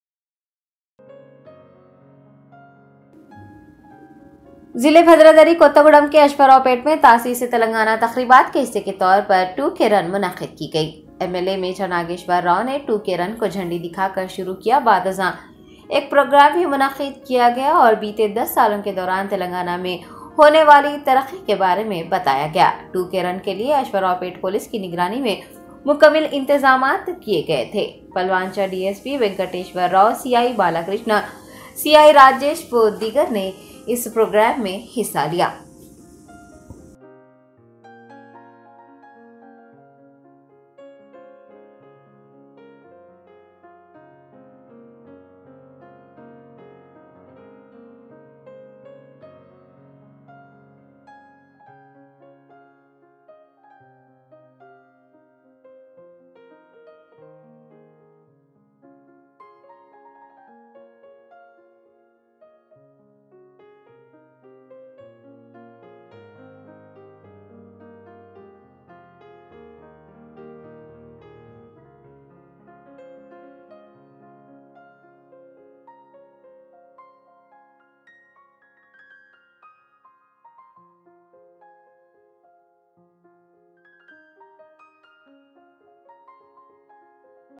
Ashwaraopet MLA M Nageshwar Rao flagged off 2k run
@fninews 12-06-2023
fni fninews II fninewsagency II fninewshydeabad II freedomnewsinternational